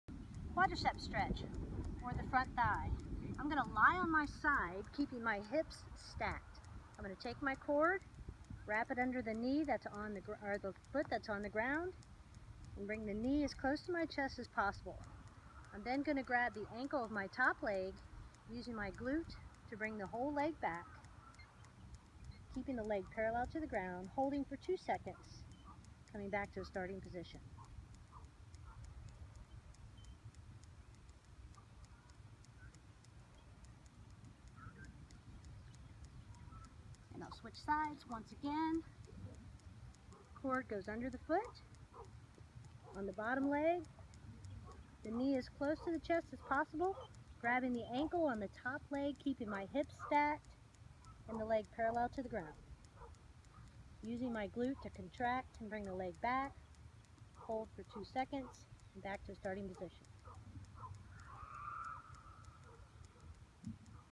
Golf Stretching Exercise Series | Lesson 9 | Quadriceps Stretch
Lie on your side, keeping your hips stacked.  Flex/bend the knee that's on the ground towards your chest (as comfortably far as your body will allow).  Place the cord under the foot and hold.. Bend the knee of the top leg and gently grab your ankle.  Contract the buttock muscle to extend the bent knee back assisting yourself with a gentle pull of the ankle holding stretch for two seconds.    Exhale on the stretch phase and return to the starting position.   Repeat 8-10 times